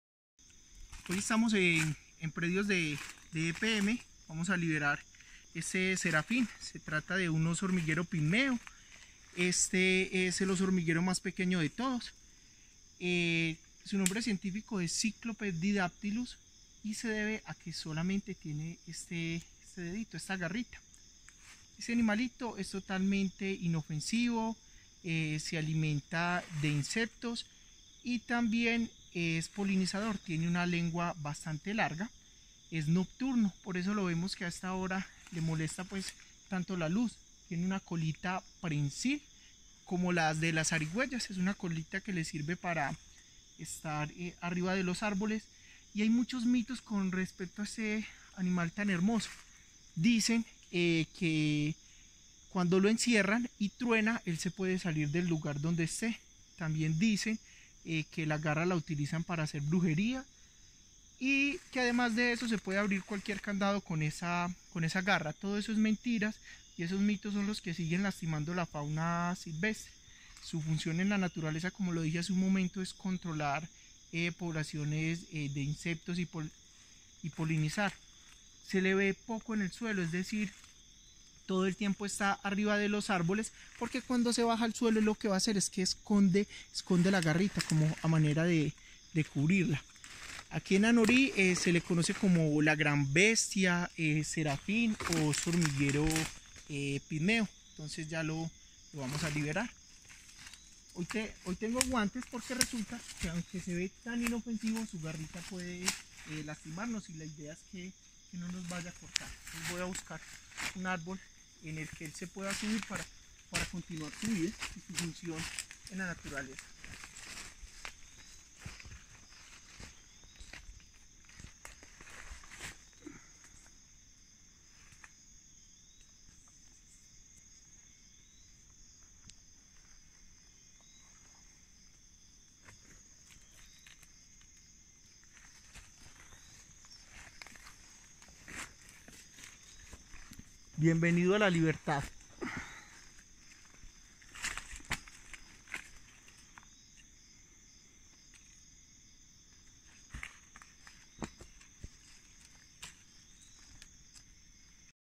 Cyclope didactylus
El hormiguero pigmeo u hormiguero sedoso es una especie de hormiguero. Anteriormente se consideraba como la única especie del género Cyclopes, natural de México, América Central y del Sur, y de la familia Cyclopedidae a la que pertenece.​